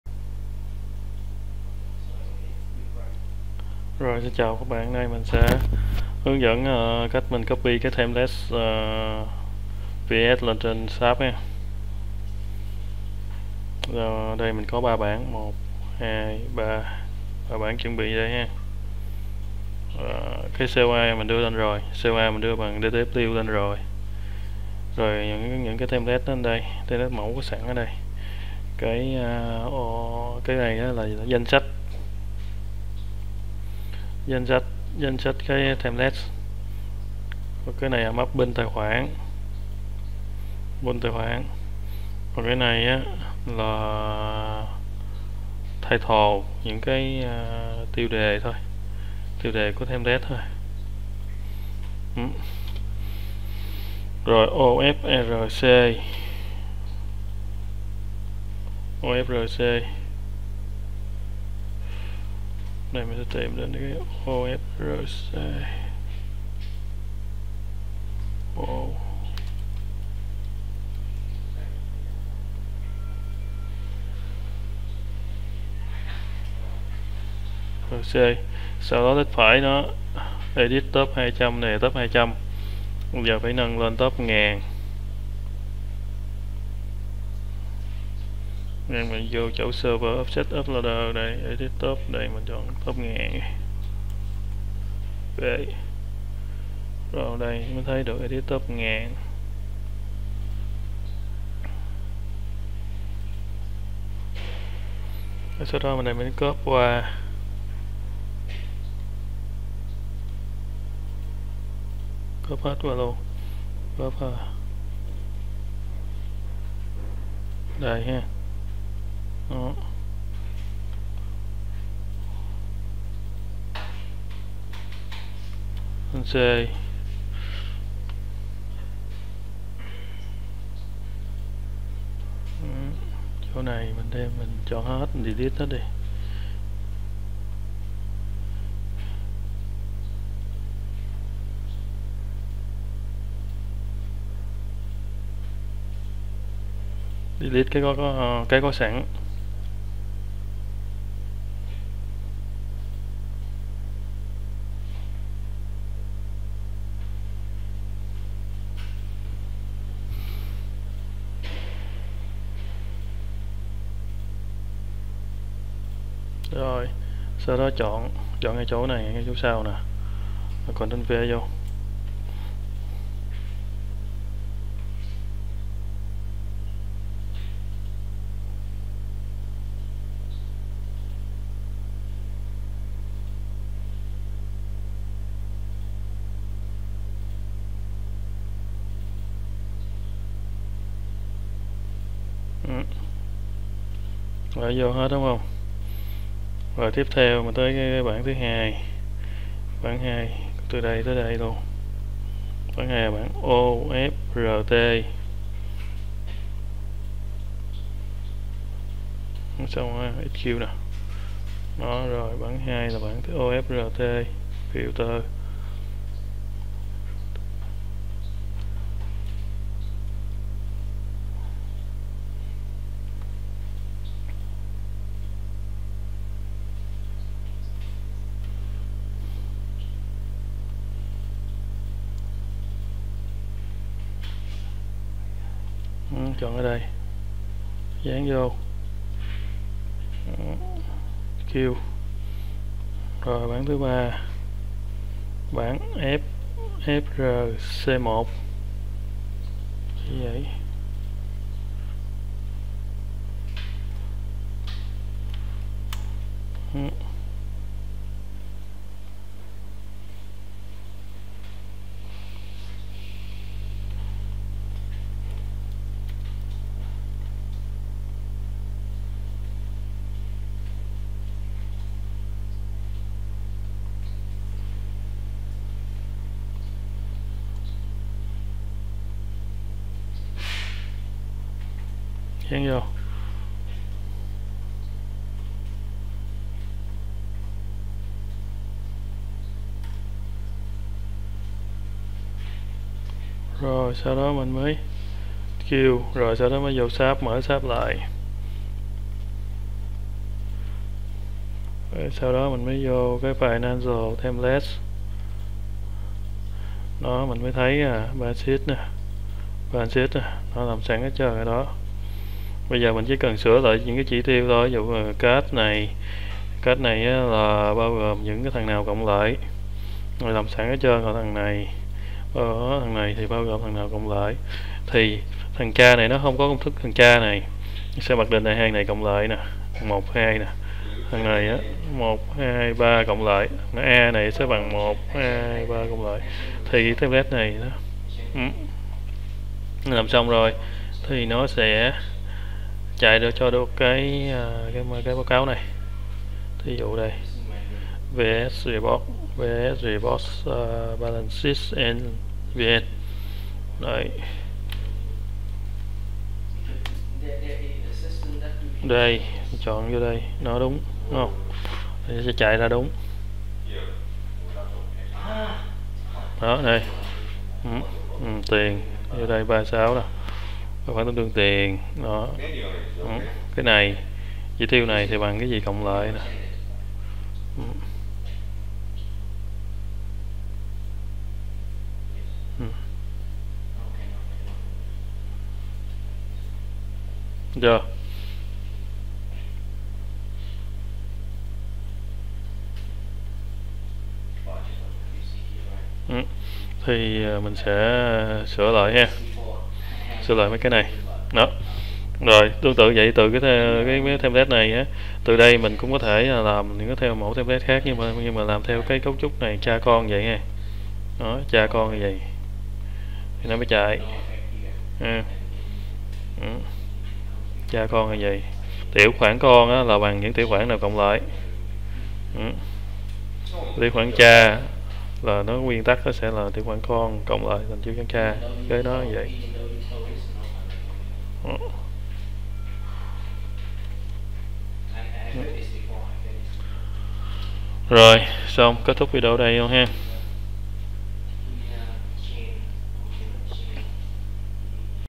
How to Import Finance Templates in SAP Business One
Learn how to import finance templates into SAP Business One with this step-by-step guide. We'll cover everything from preparing your template files to successfully importing them into the system, ensuring your financial data is correctly structured and ready for use. This tutorial is ideal for finance professionals and SAP Business One users looking to streamline their financial processes.